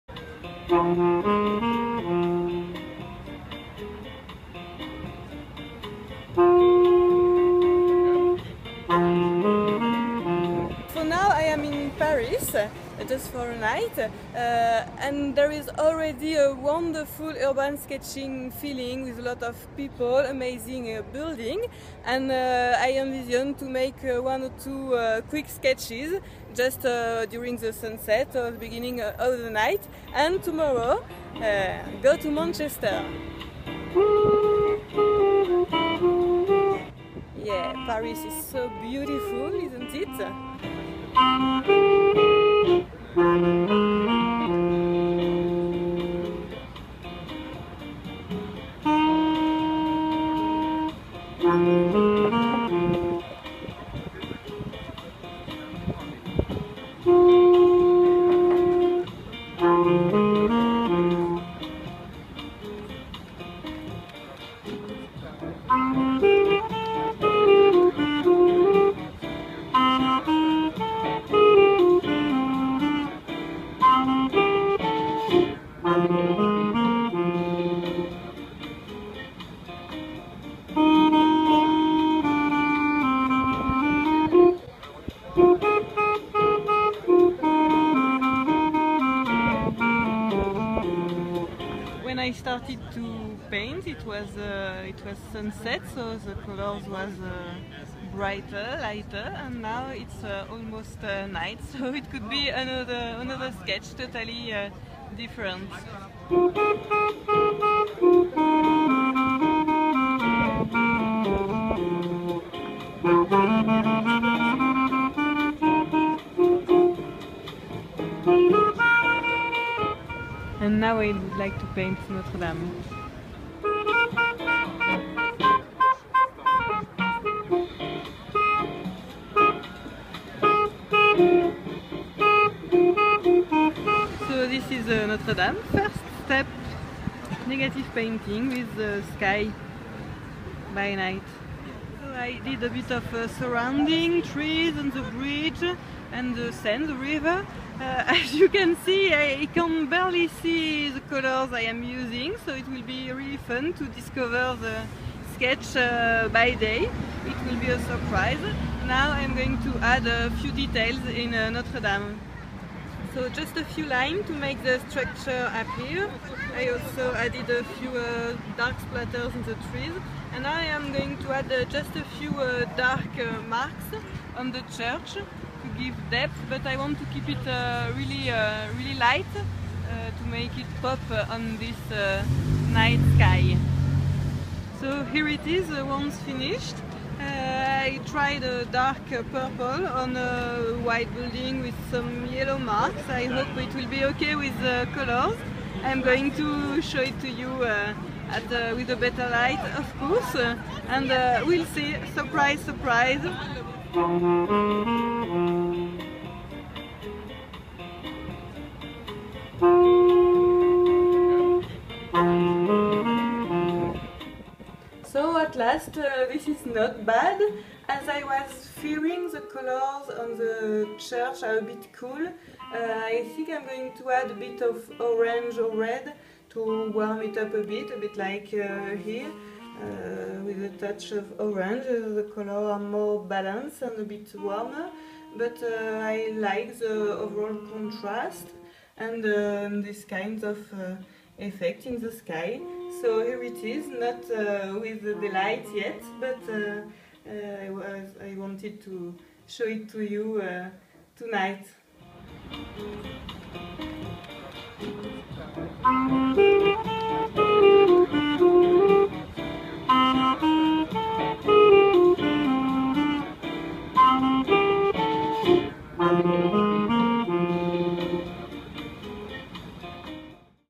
NOTRE DAME DE PARIS Watercolor. URBAN SKETCHING in Paris!!
I am painting Notre Dame de Paris on location, Watercolor Urban Sketching Style and sharing with your the painting process step by step. One of my first Urban Sketching VLOGS!! 😊

This is an "atmosphere video" with some shots of Paris during the sunset and by night and a watercolor step by step. I painted two impressive buildings, especially the famous "Notre Dame de Paris", on Canson Montval Paper.